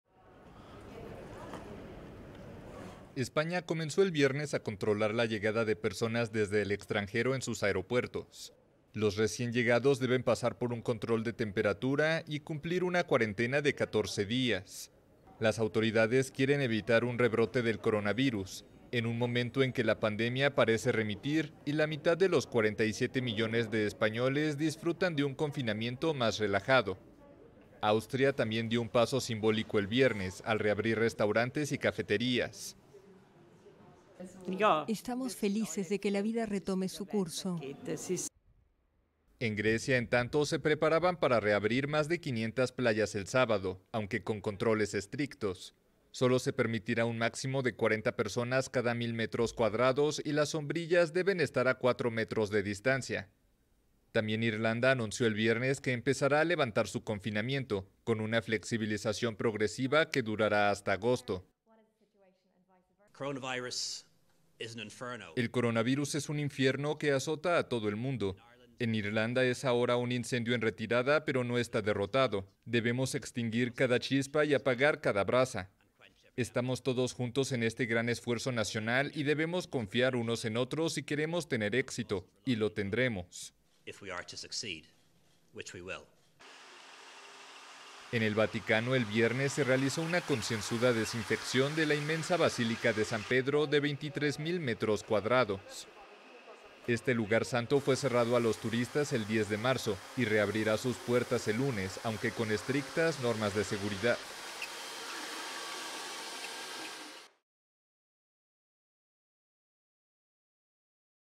Cafés abiertos, controles en aeropuertos, desinfecciones...Europa avanza en cauteloso
Austria abrió el viernes restaurantes y cafeterías y España comenzó a controlar la llegada de personas desde el extranjero en sus aeropuertos, mientras Grecia se prepara para abrir sus playas y el Vaticano la basílica de San Pedro, todo bajo estrictas normas de seguridad.